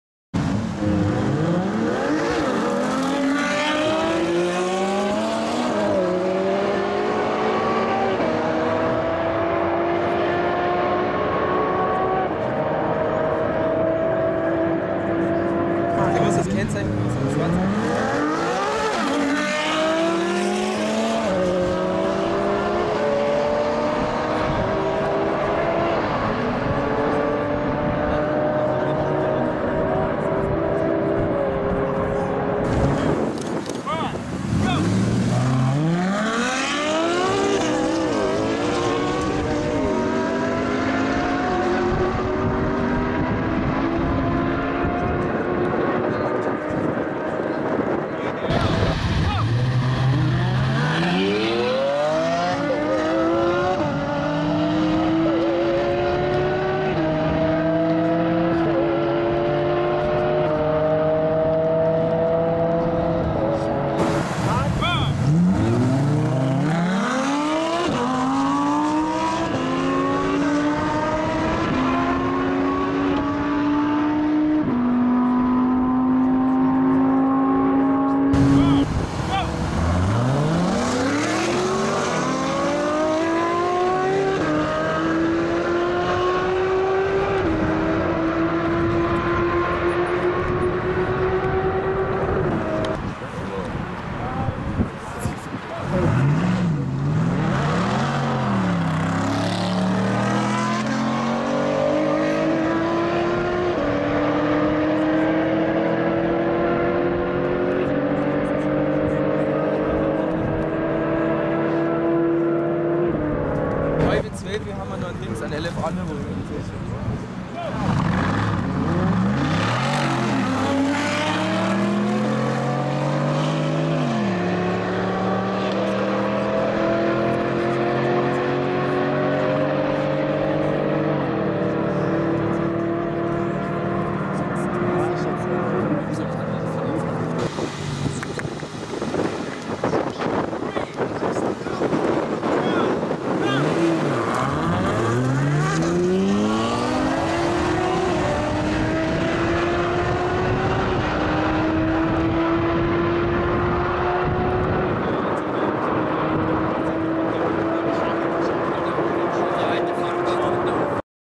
DRAG RACE Ferrari F12 TDF vs 720S vs Huracan Performante vs Aventador & others!
I filmed a drag race of the Mclaren 720S vs the Ferrari F12 TDF and other supercars and hypercars! Filmed during the Supercarownerscircle event in Andermatt.